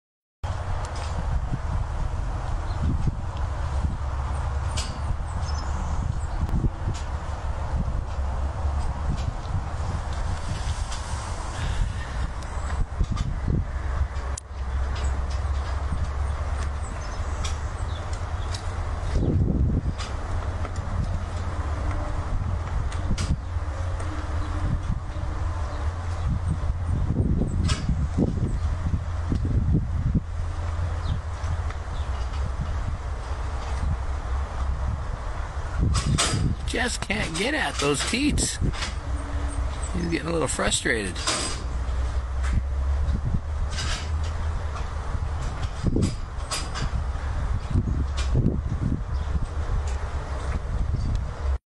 עגל מנסה לינוק עם מחסום
תעשיית החלב לא רוצה שחלב האם "יתבזבז" על עגל תמים שרוצה לינוק מאמא שלו.